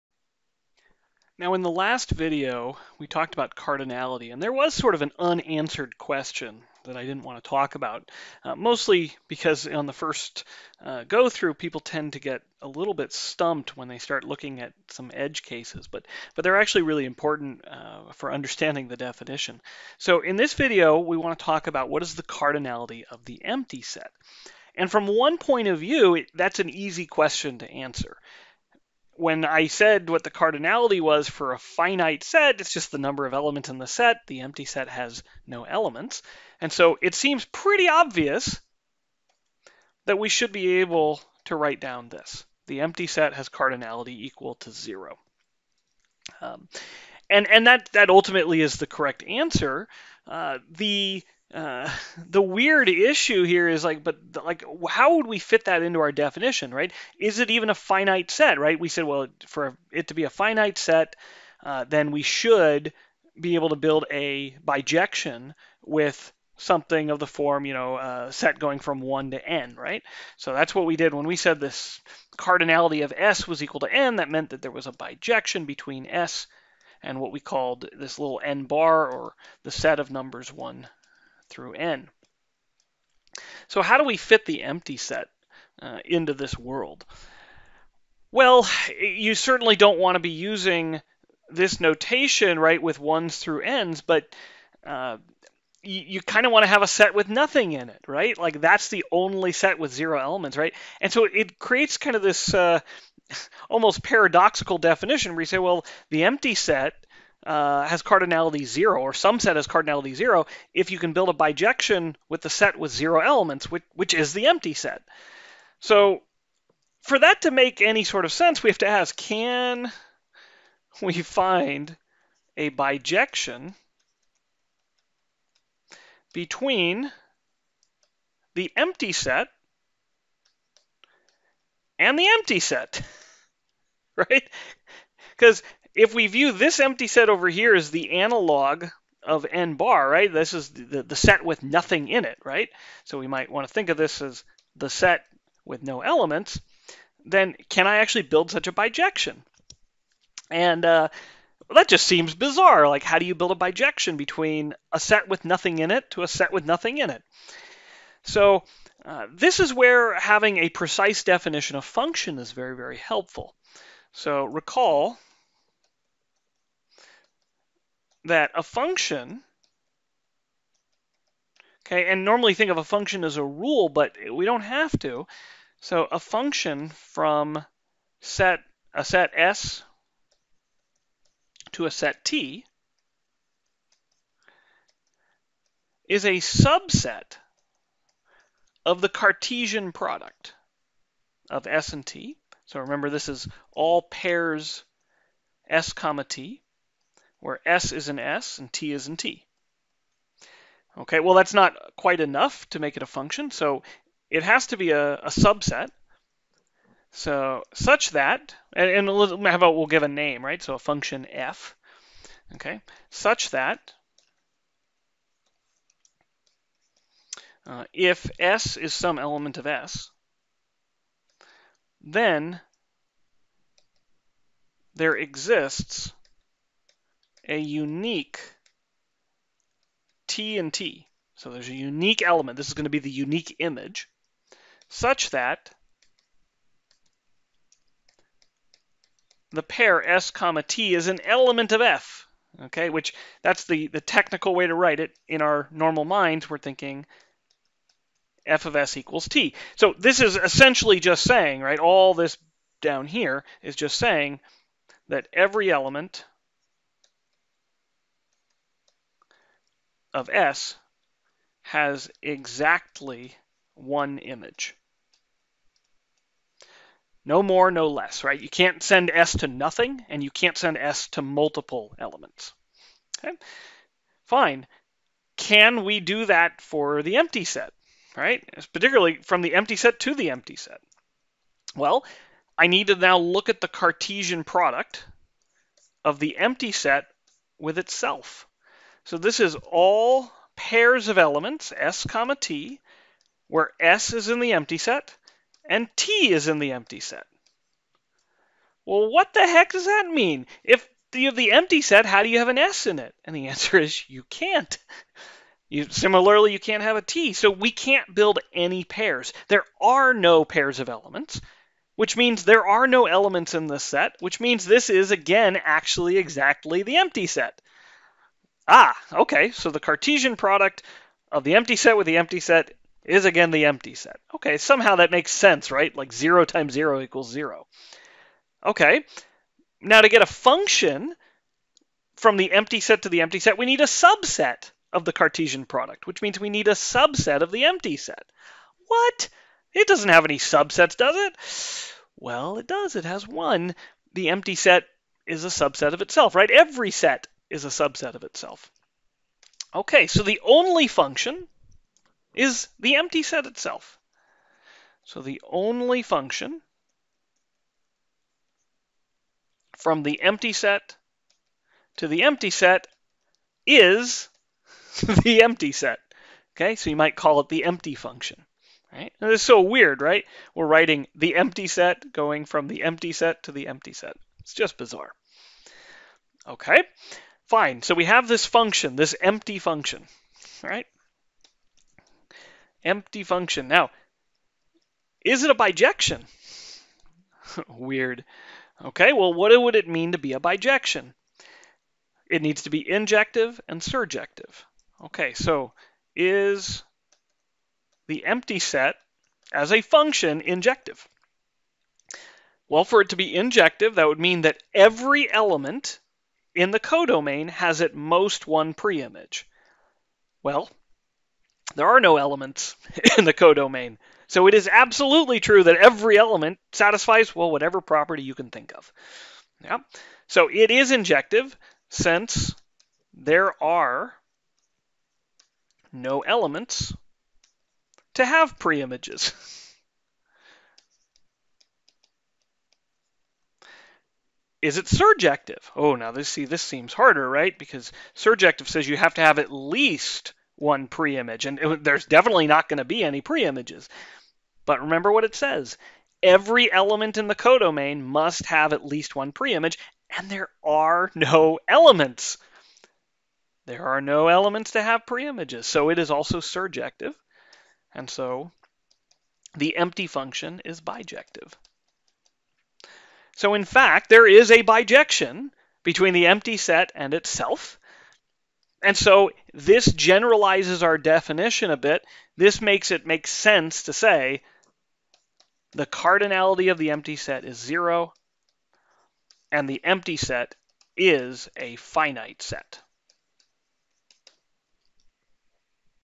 Cardinality of the Empty Set
We discuss cardinality of the empty set. In particular, we explain why it makes sense to assign it the cardinality of 0 and to call it a finite set.

0:00, Intro
0:34, Cardinality Definition
2:40,Recall Function definition
4:35, Bijection from the empty set and itself